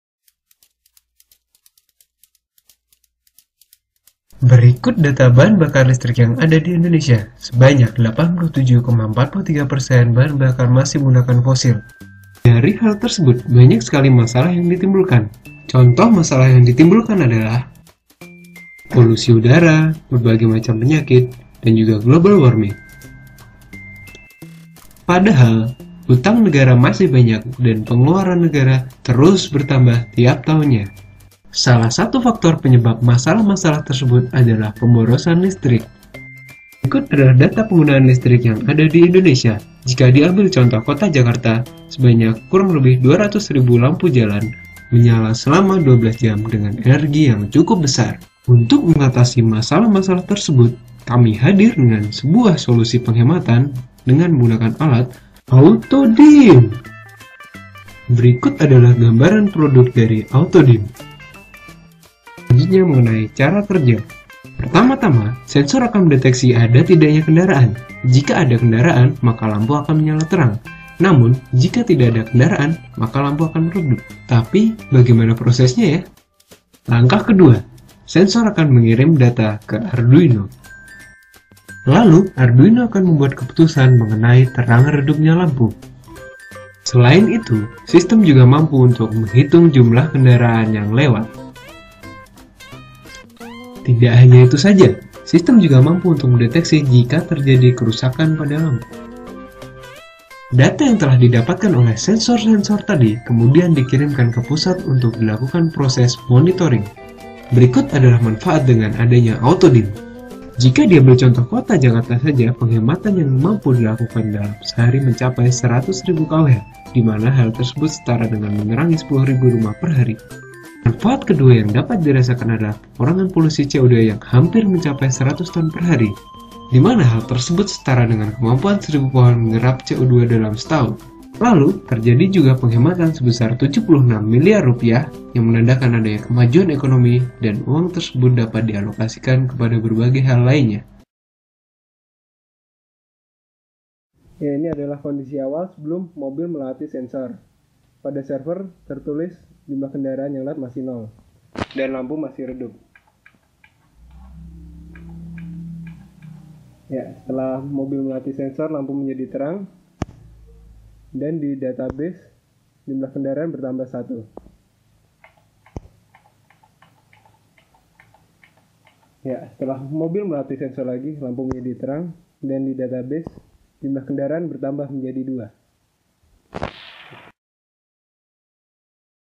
Autodim 3.0 IOT Innovation Challenge 2015
a video tell about the design of Automatic Lighting Street Dimmer and Monitoring system using ESP8266 and Arduino Uno